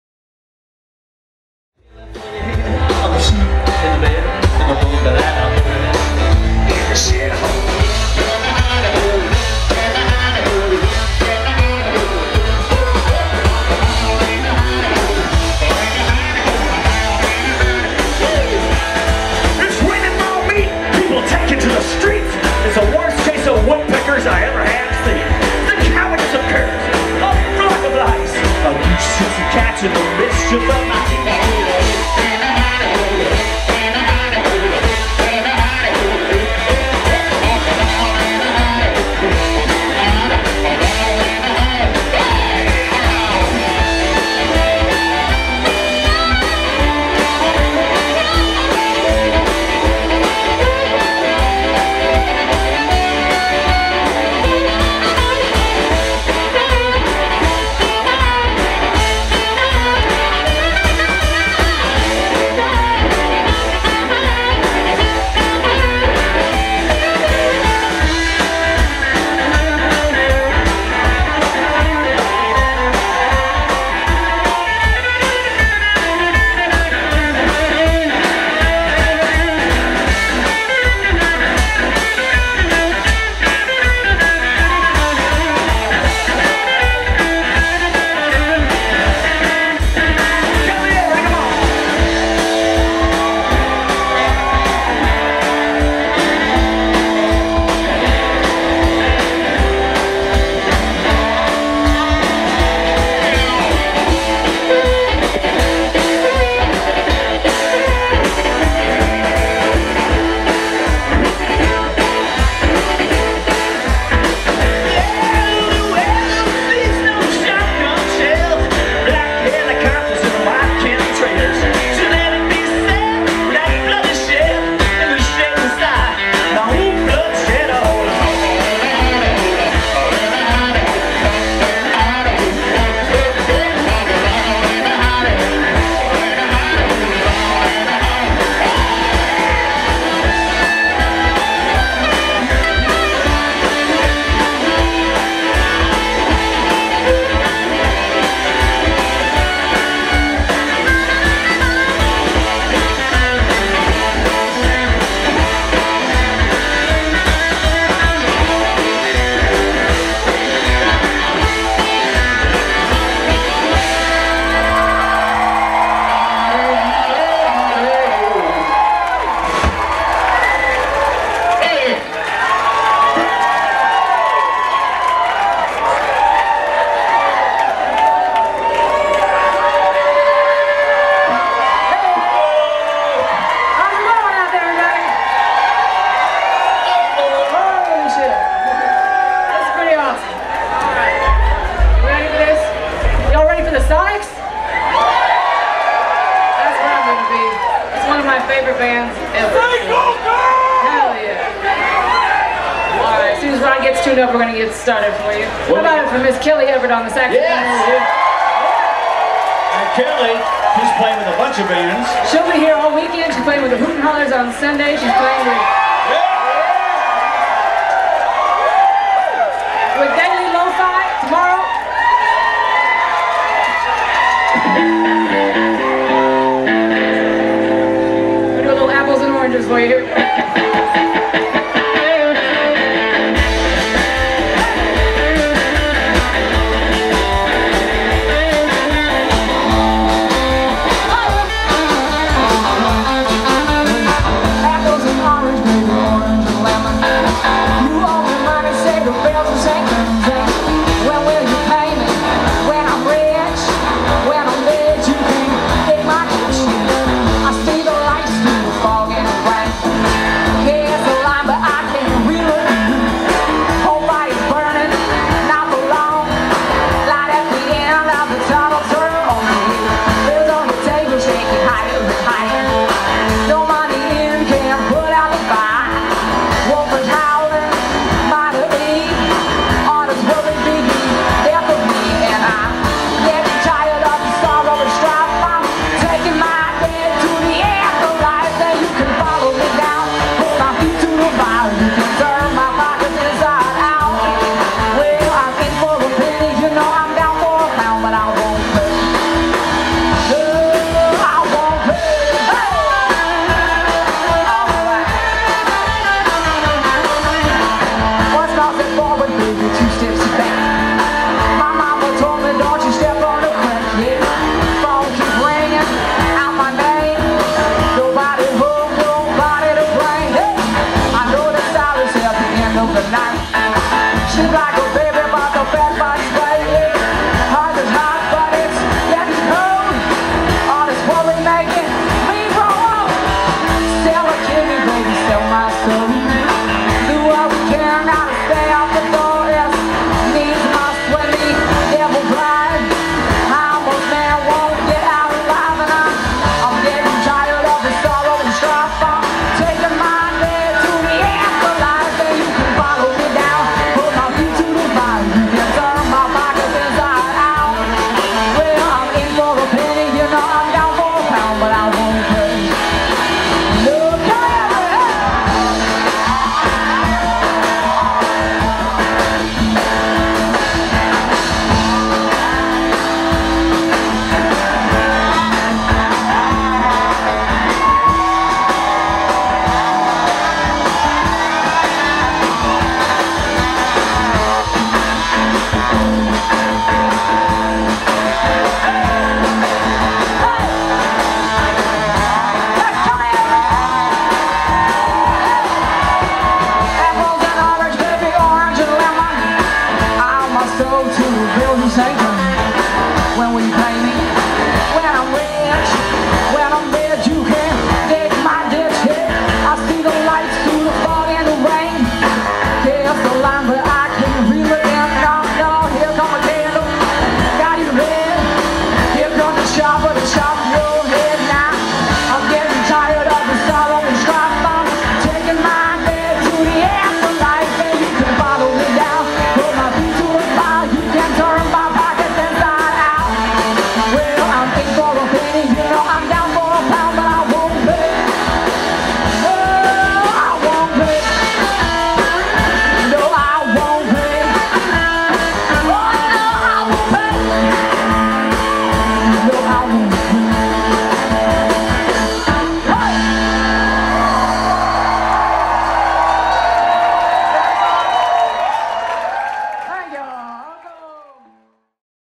Legendary Shack Shakers: 8/29/14 Muddy Roots Music Festival, Cookeville, TN
The Legendary Shack Shakers perform live at Muddy Roots Music Festival in Cookeville, Tennessee on August 29, 2014